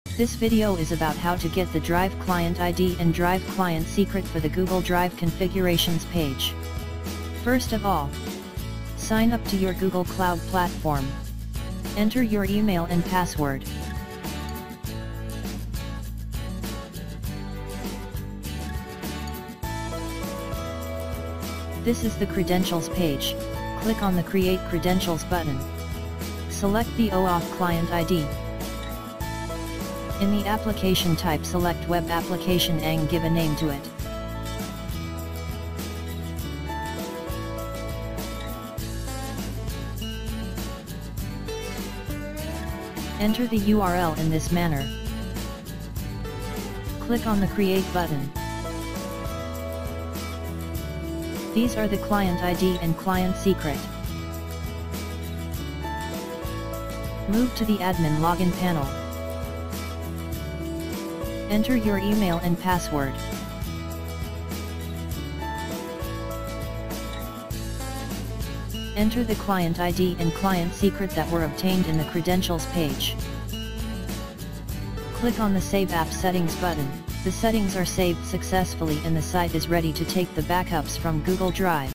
How to generate and use API keys For Google Drive?
This video presents the working of the plugin that ensures safe protection of the Wordpress sites and it's data security, in just one click.
-A huge amount of time and hard work is devoted by you on your website but if it's not bounded by the safety measures, it leads to intolerable dangers. Therefore, the plugins help you to get the regular auto backups that remove your worry to lose data and information in case of site changes and software up-gradation.
-This extension will extremely take care of the backups and let you restore your site in just a one-click which will eventually ensure a high degree of the data security on your website in the long run.

Benefits-
1. You can shield your website from getting a sudden crash.
2. Clone WordPress via Google Drive
3. You can still take backups of selected files and folders for future use.
4. By simply scheduling it, no need to take regular backups.
5. Restore any backups for future use.

1. For better and safe backup, restore clone for WordPress via Google Drive, you need to first go to WordPress site for backup.
2. There you will find two peripherals like a local server and via Google Drive.
3. You can now easily create a backup of the WordPress from the address you had installed.
4. From there, you can restore the information/file/folder for future use.

[Hashtag Section]